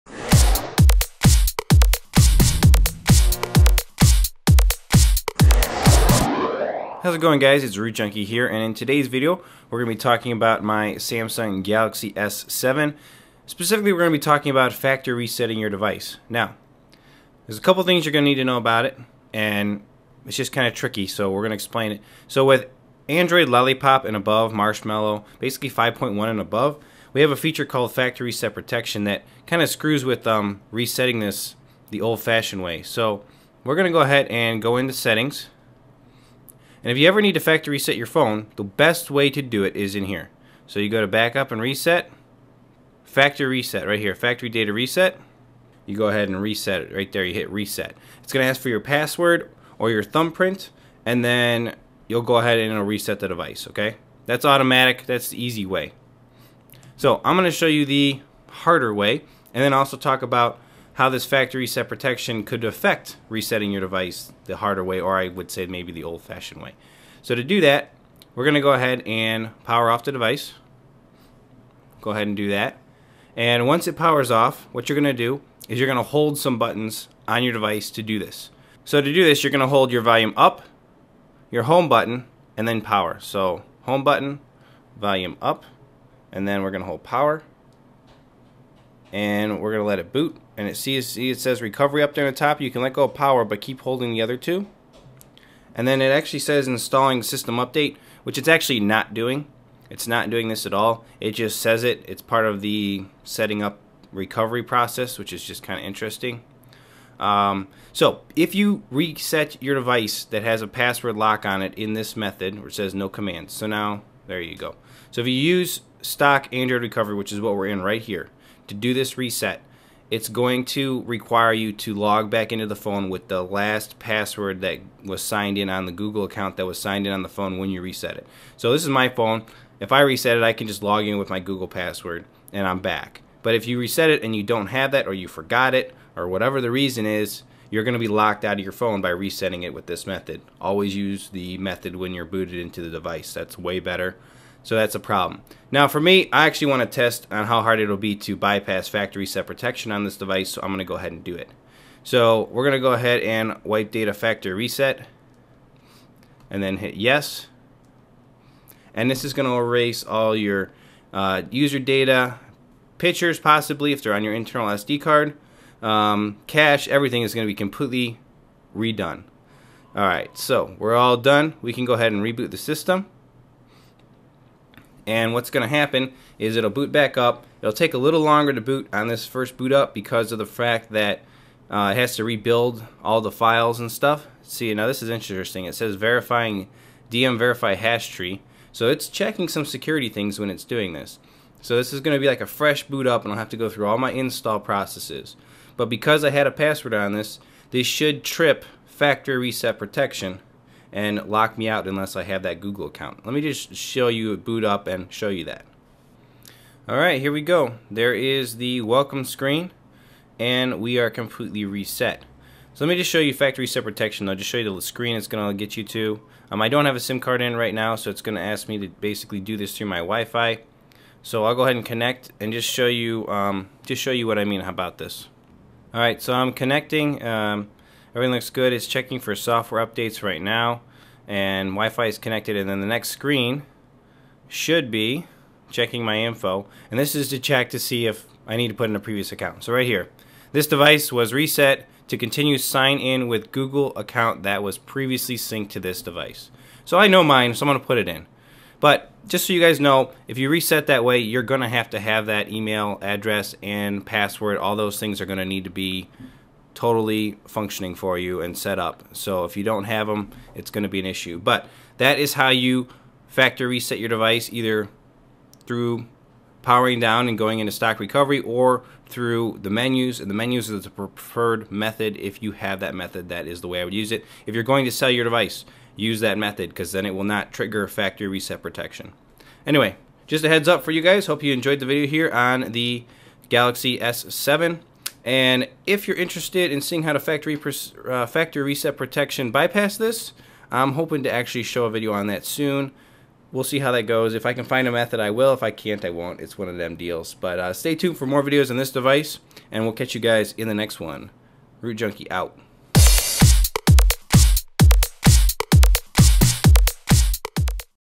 How to Factory Reset the Samsung Galaxy S7 or S7 Edge
How to wipe data Factory Reset the Samsung Galaxy S7 or S7 Edge.